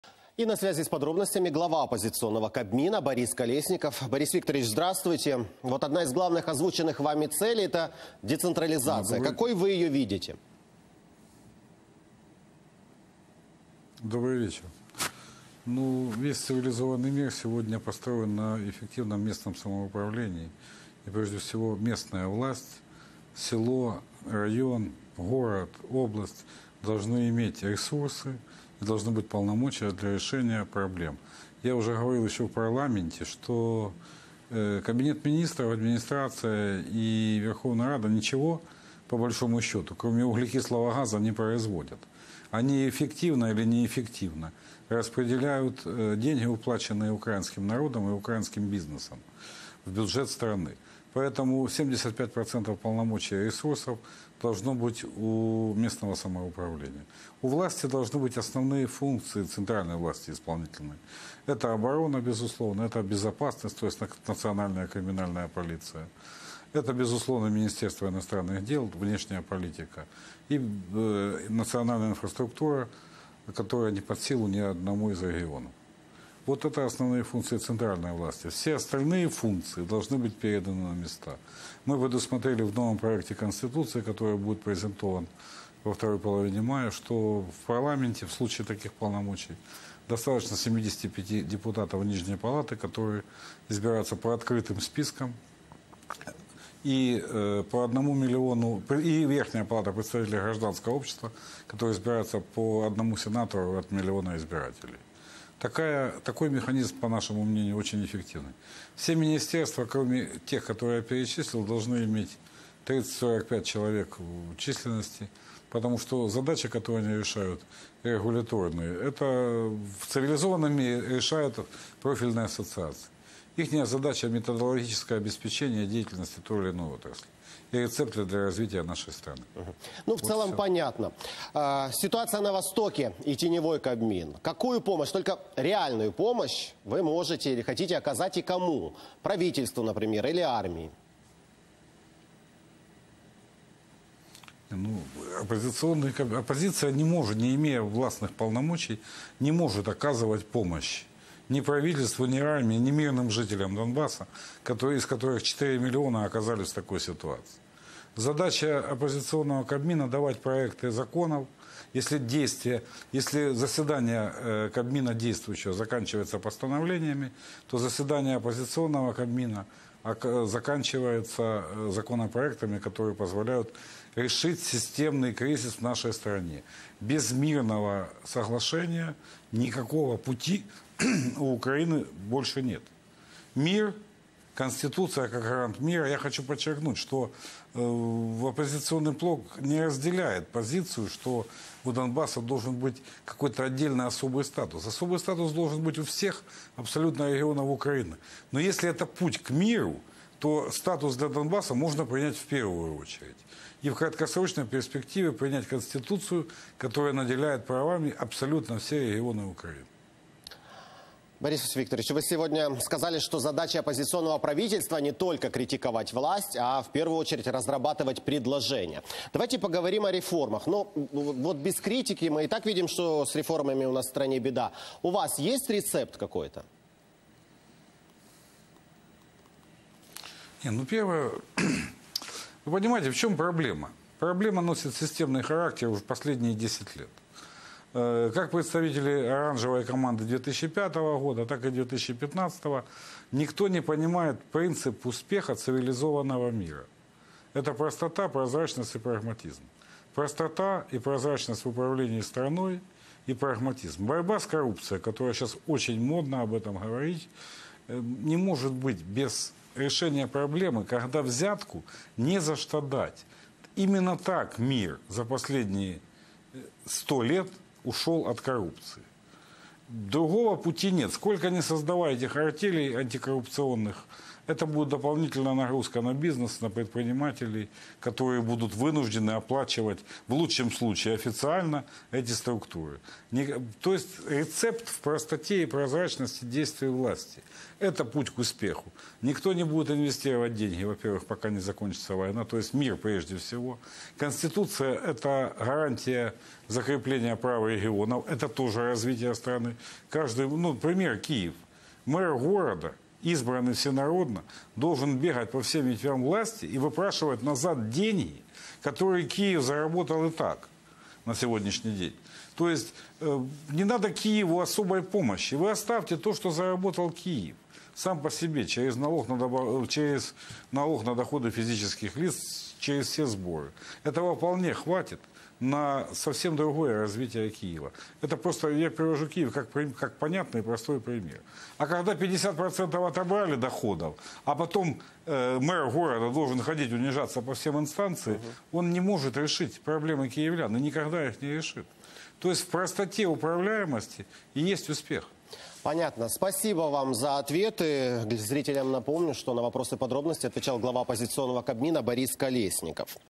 Борис Колесников: Оппозиция презентует новую Конституцию
Оппозиция готовит новый проект Конституции, предусматривающий децентрализацию в Украине. Оппозиция не имея властных полномочий не может оказывать помощь ни правительству, ни армии, ни жителям Донбасса.